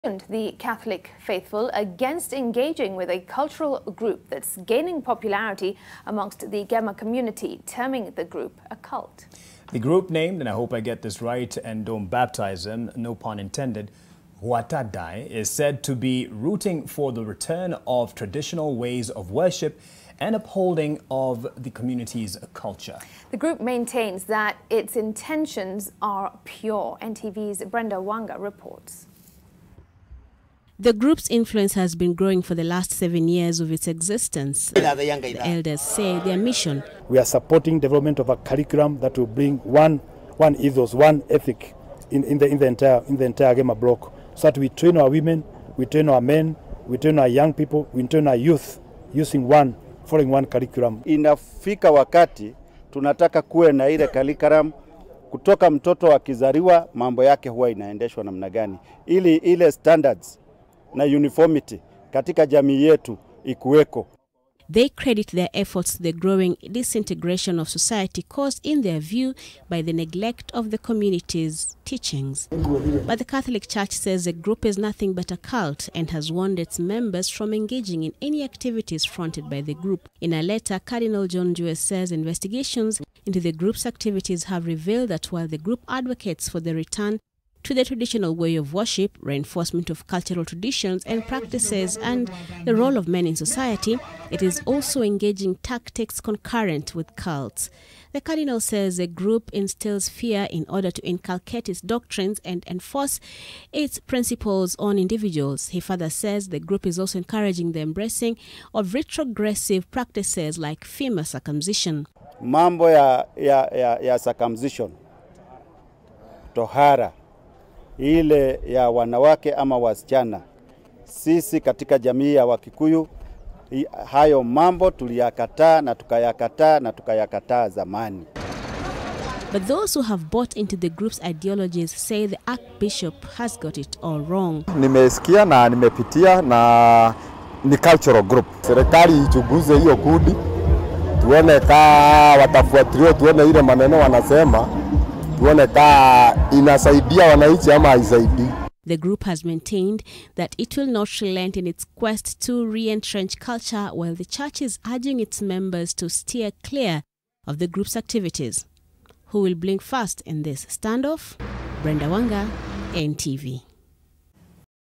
Cult? Cardinal John Njue cautions Catholics against 'Gwata Ndai' grouping
Cardinal John Njue has cautioned the catholic faithful against engaging with a cultural group that is gaining popularity amongst the GEMA community.

Get the Kenya news updates, discussions and other exciting shows.